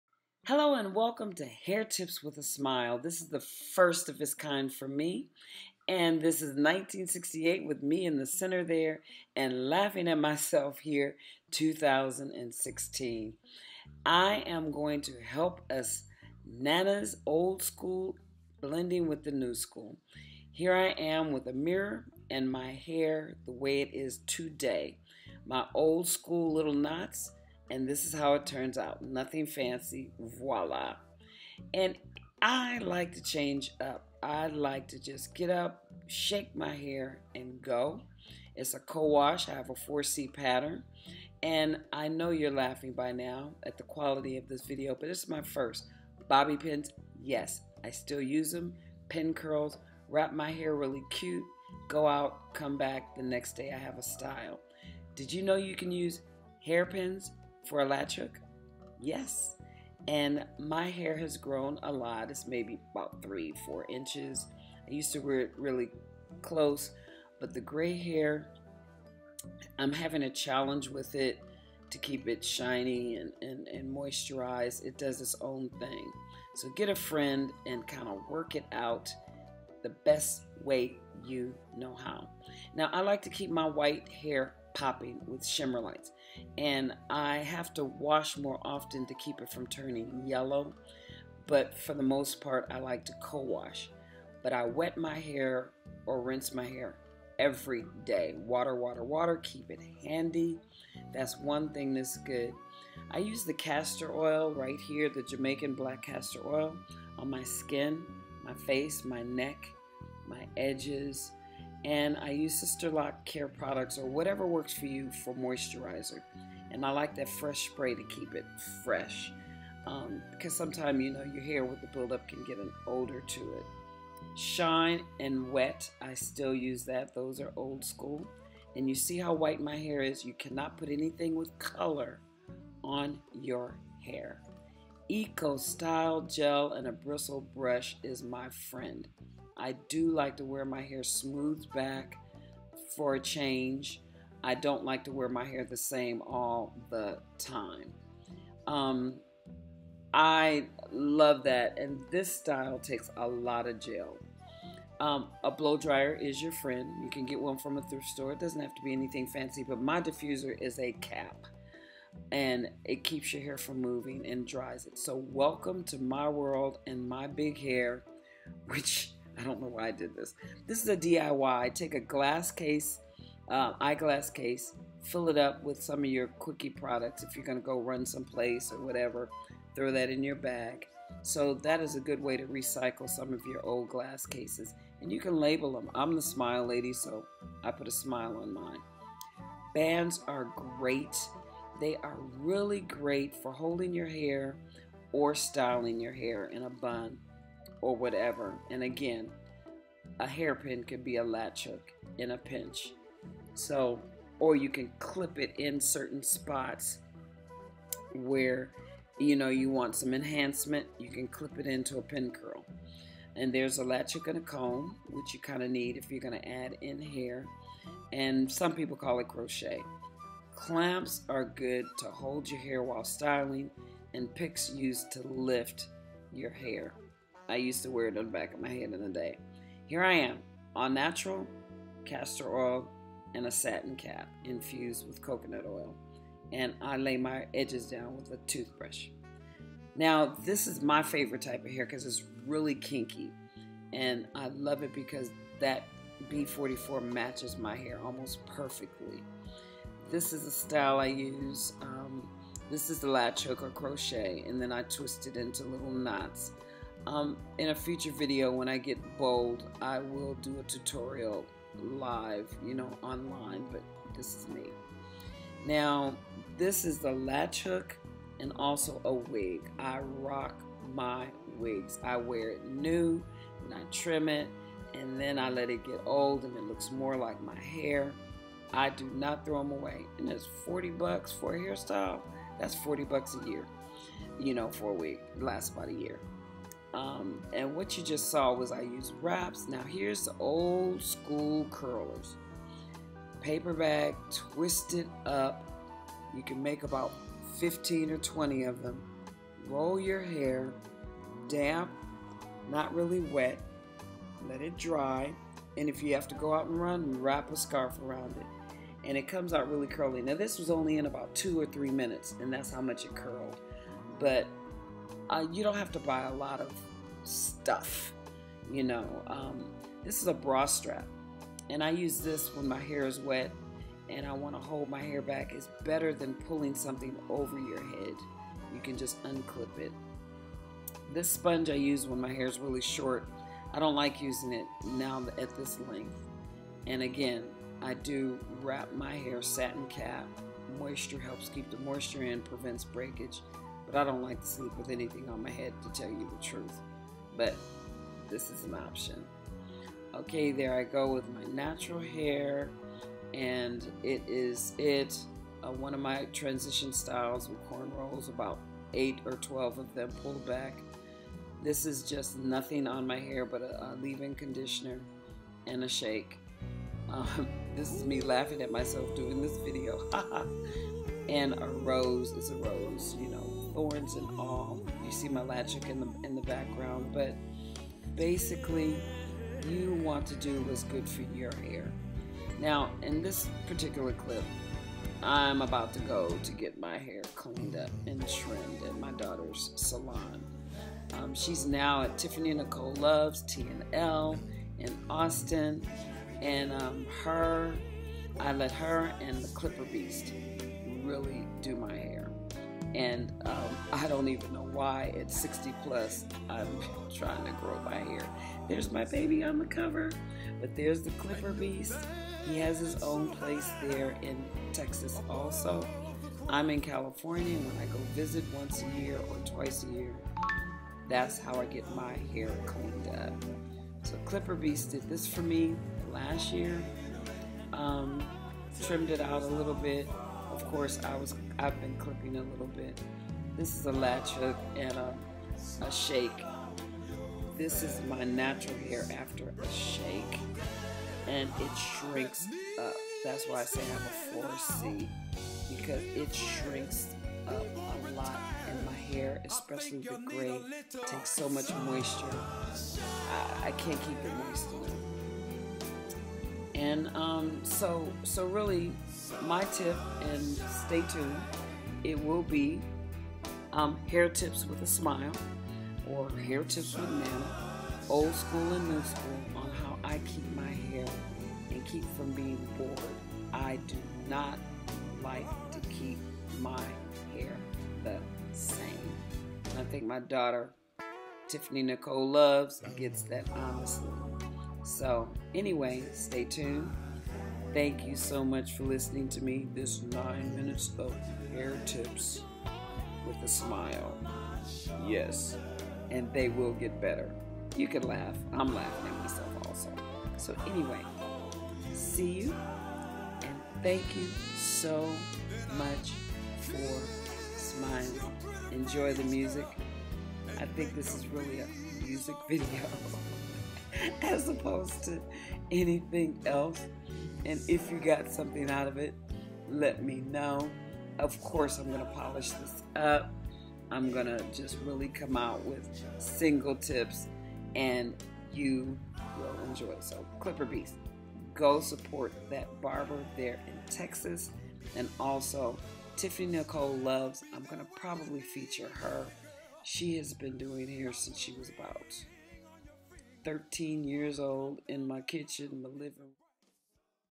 SMILEandSpreadaLittleJOY! Hair and Beauty Tips #1 With A SMILE
SMILELaDy's First Video  Sharing  HAIR Tips, Very RAW a work in Progress, T Having Challenges Working with grey Hair (Tips Welcome) , Transition Styles Old School and New,  DIY Tips.   Will Post Step By Step Proess in the future.